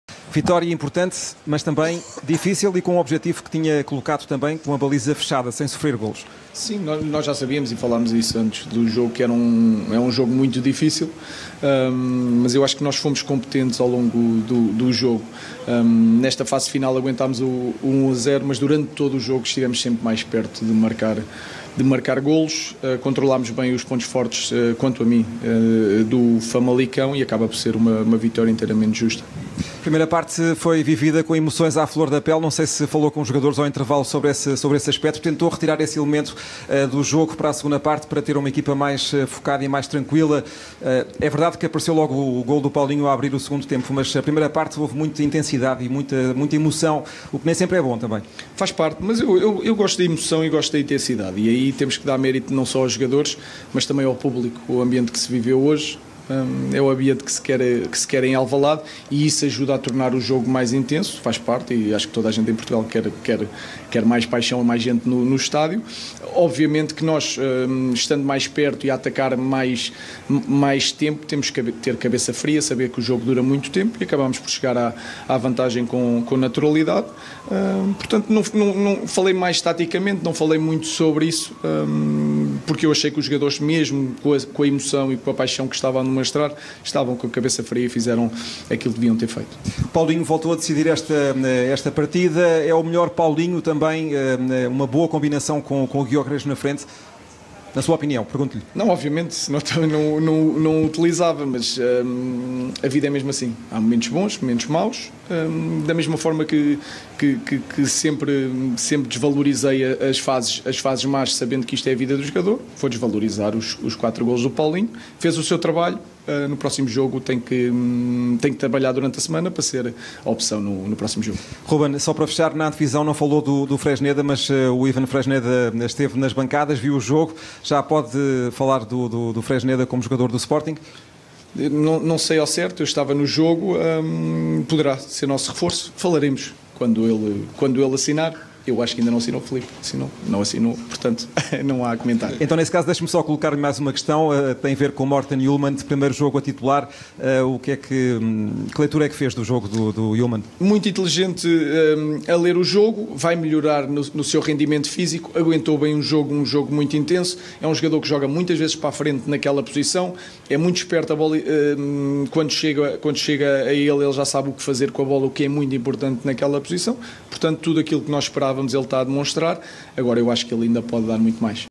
Rúben Amorim: "Fomos competentes"
Rúben Amorim disse que a sua equipa foi competente, esteve sempre mais perto de marcar e por isso o triunfo é justo no seu entender. Quanto a Fresneda, reforço que estará prestes a assinar pelo Sporting CP, o treinador não comentou a sua presença em Alvalade uma vez que o negócio ainda não estará fechado.